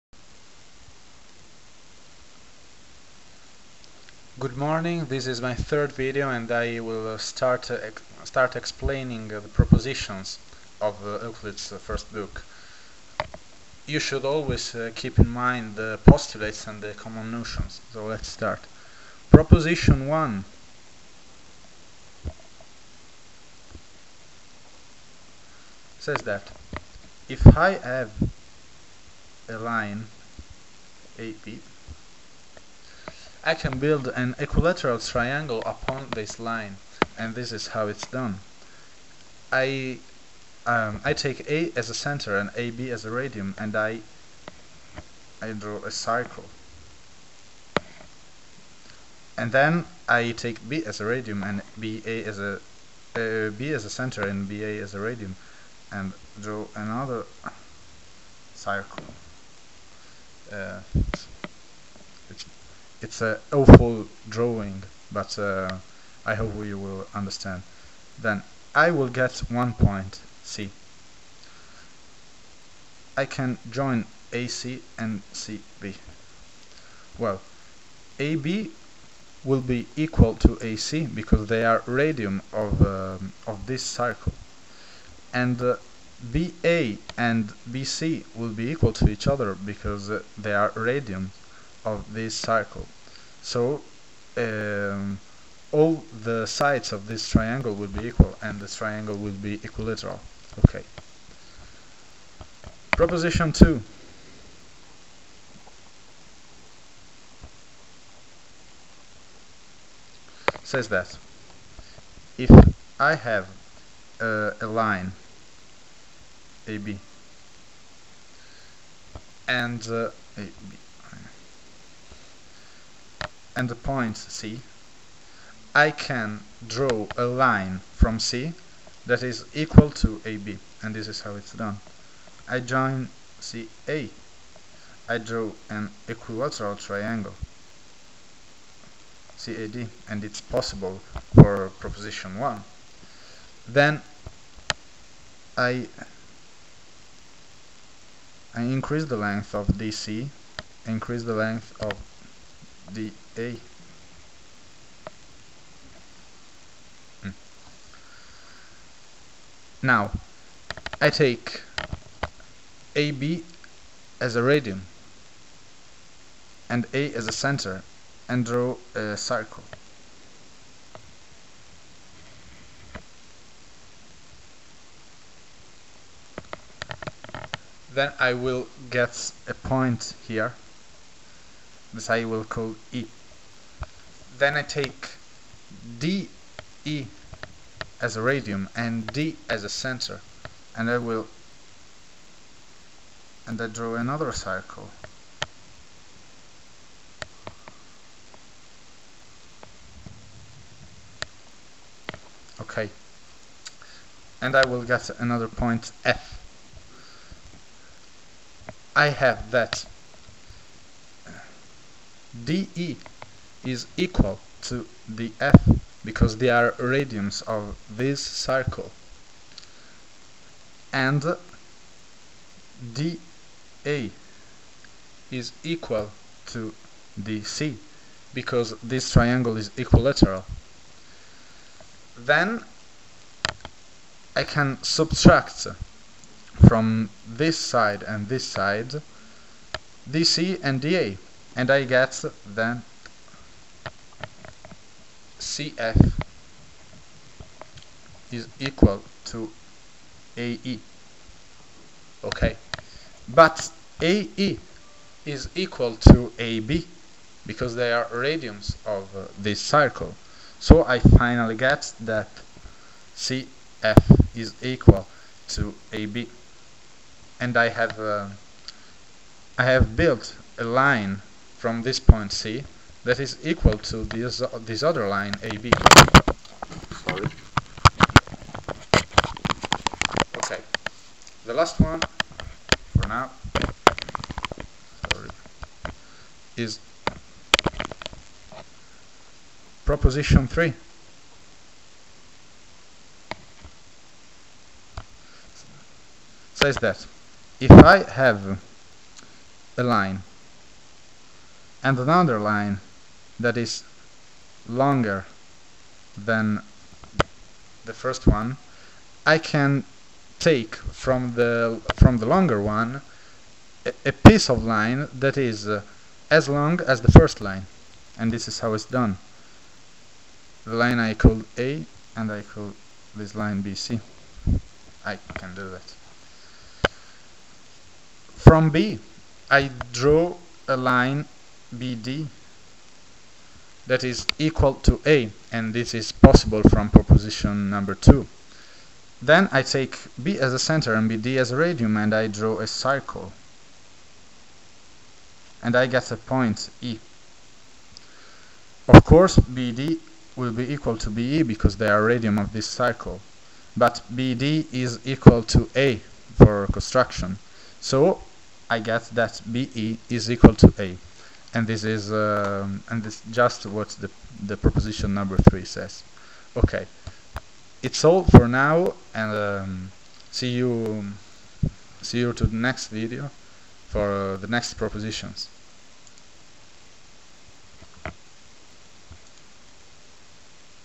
Euclid's Elements: Propositions 1-3
This is my third video, in which I explain the propositions 1-3 of the first book of Euclid's Elements. Again, sorry for my bad english.